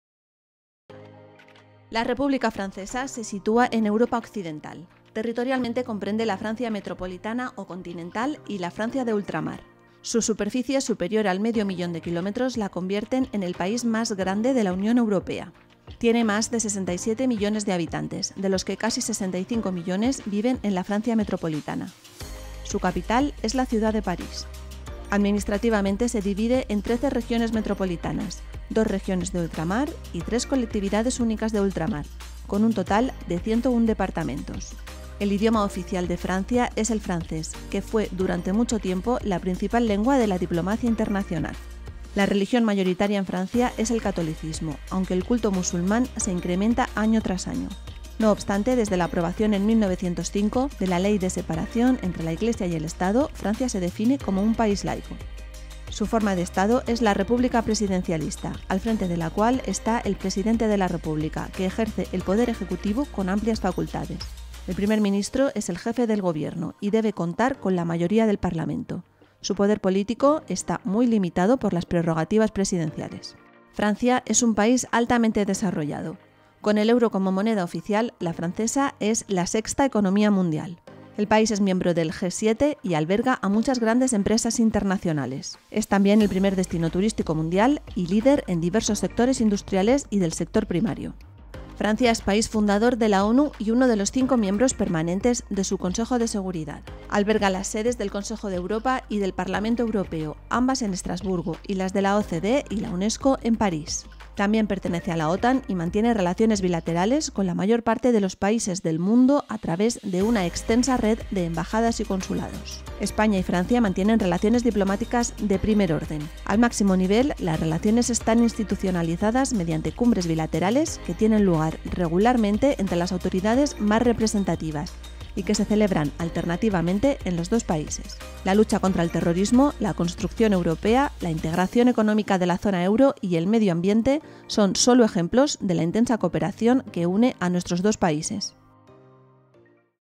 Bienvenid@s al Mediterráneo - Francia 🇫🇷
Desde Casa Mediterráneo queremos profundizar y ampliar el conocimiento sobre los diferentes territorios que conforman la región mediterránea.

🇫🇷 Hoy os presentamos Francia, el país más grande la Unión Europea, ubicado en la parte occidental del continente. Os invitamos a conocer este país a través del vídeo que hemos preparado.